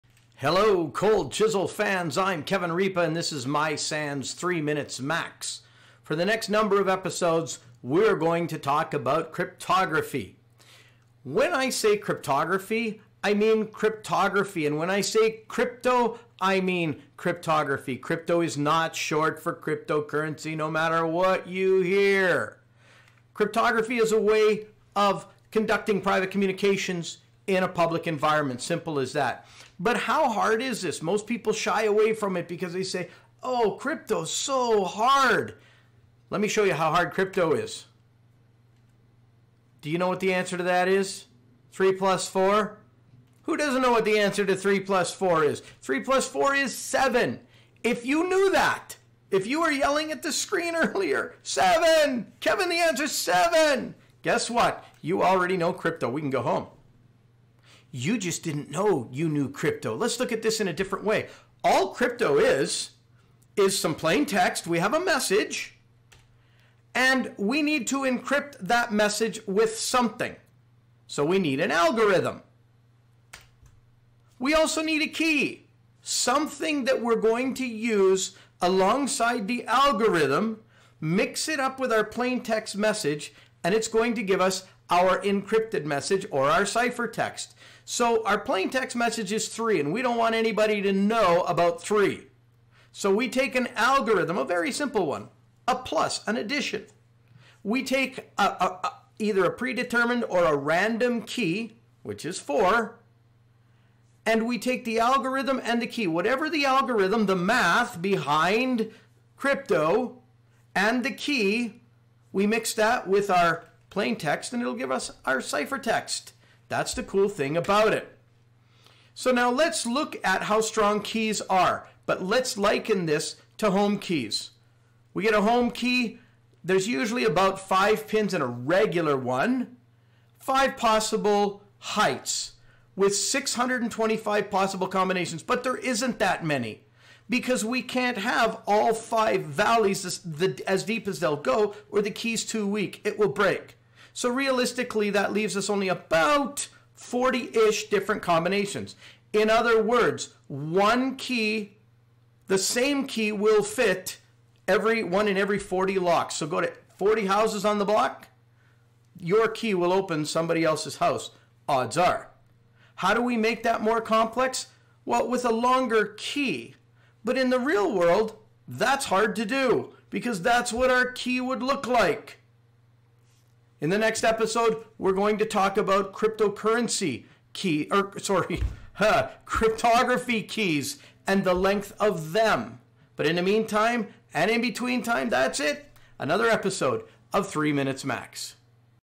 Episode 125: Encryption & Crypto - Part 1
In this initial episode, we introduce the building blocks of cryptography and key length.